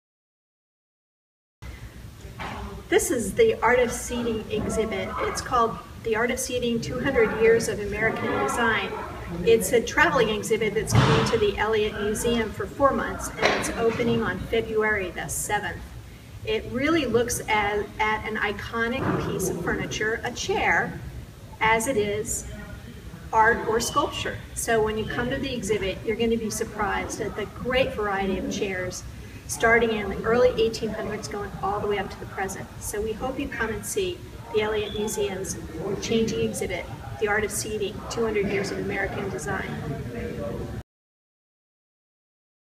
"The Art of Seating" coming to Elliott Museum
The Elliott Museum President Jennifer Esler welcomes you to the new exhibit, "The Art of Seating," in this video.